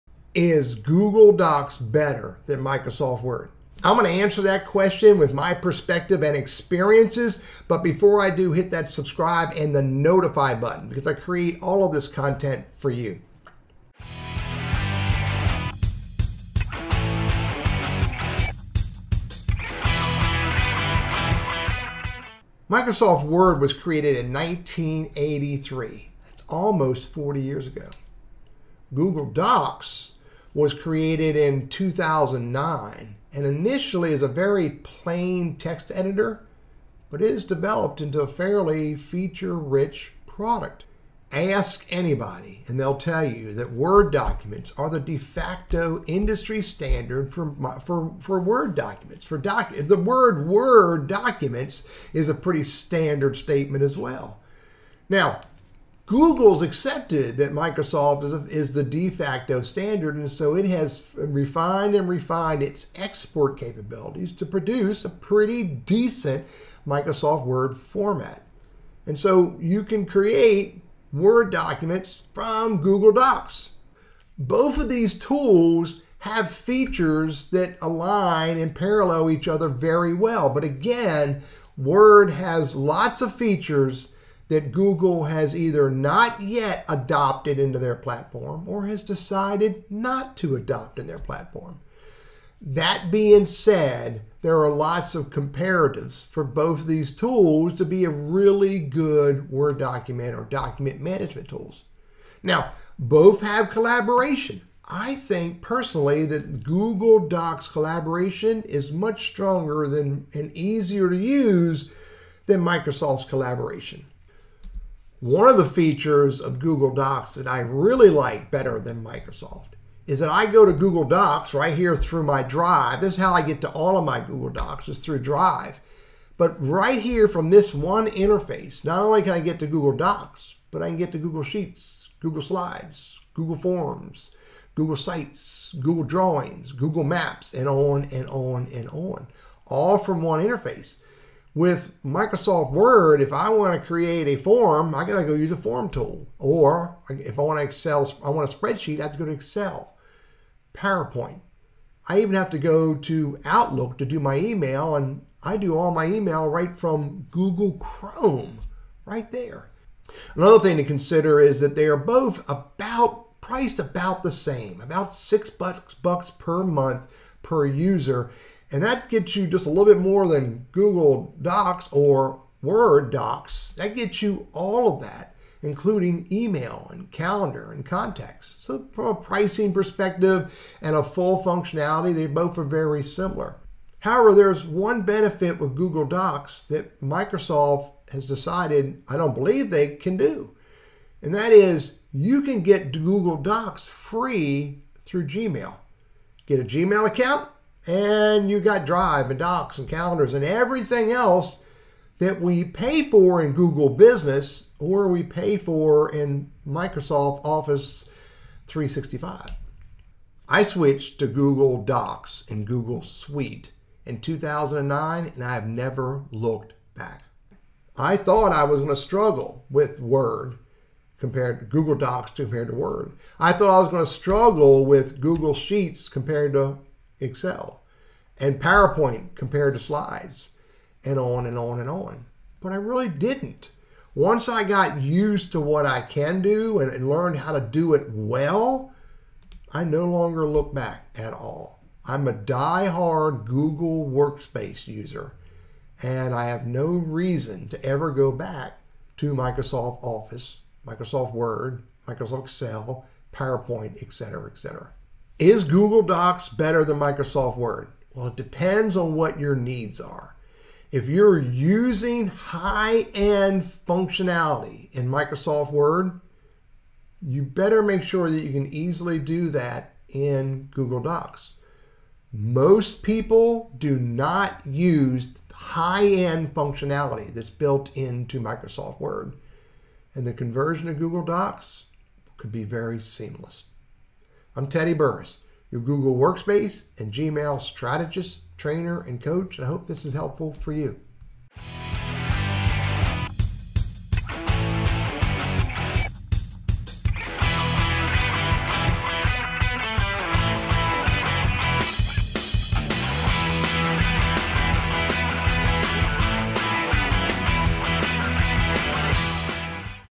Is Google Docs better than Microsoft Word in Google Workspace or Gmail?
I share my opinion and experiences converting from Microsoft Word to Google Docs.